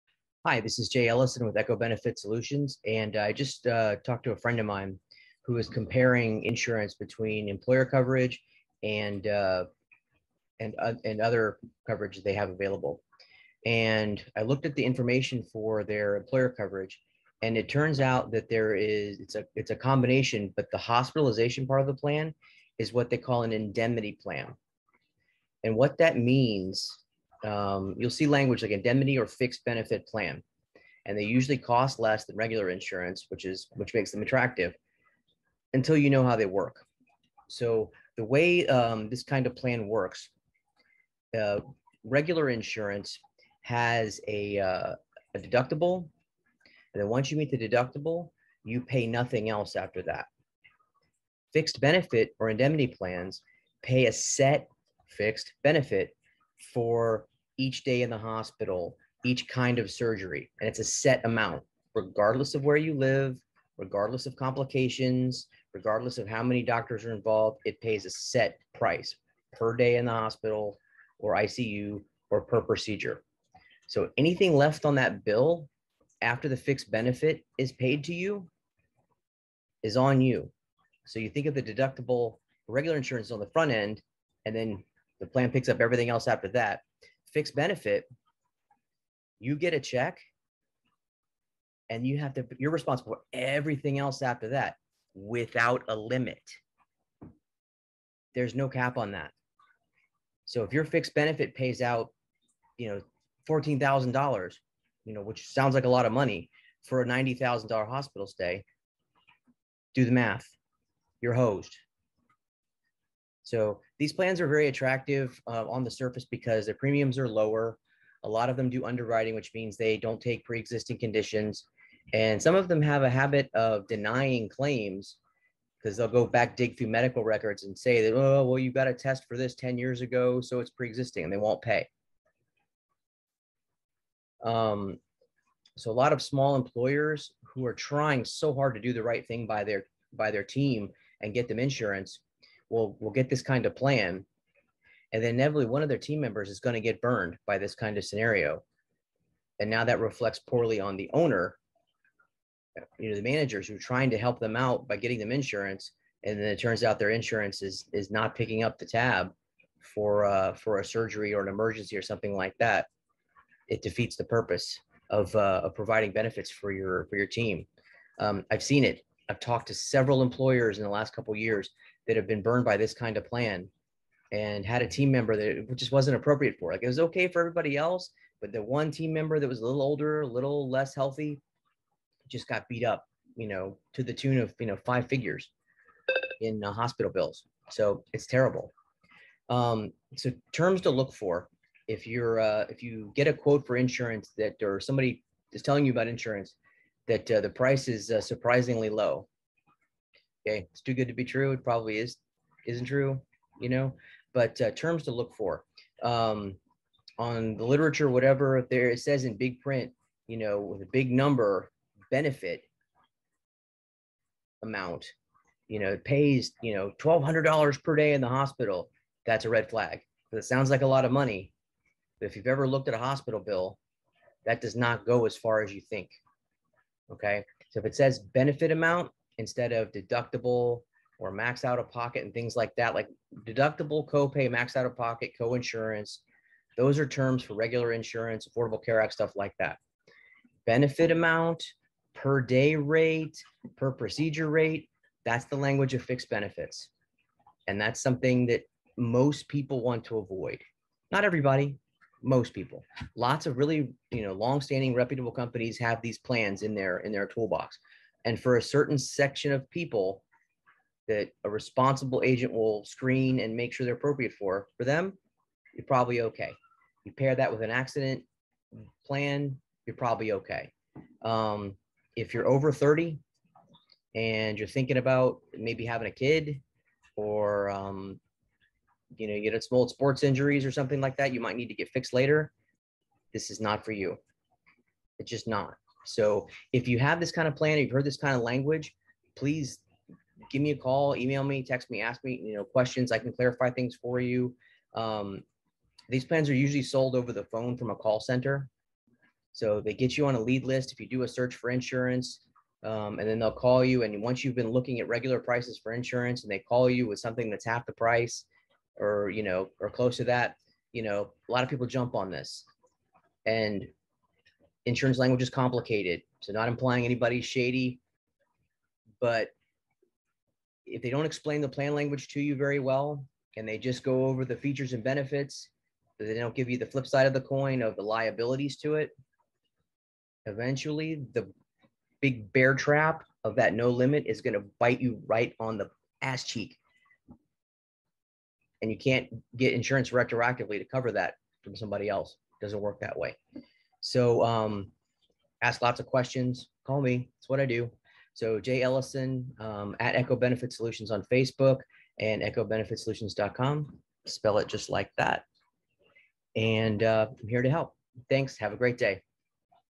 Indemnity Insurance beware
Do you know what medical/hospital indemnity insurance is? You might already have it and NOT KNOW! And it might come back to BITE you later if you're not clear on how it works and pays benefits!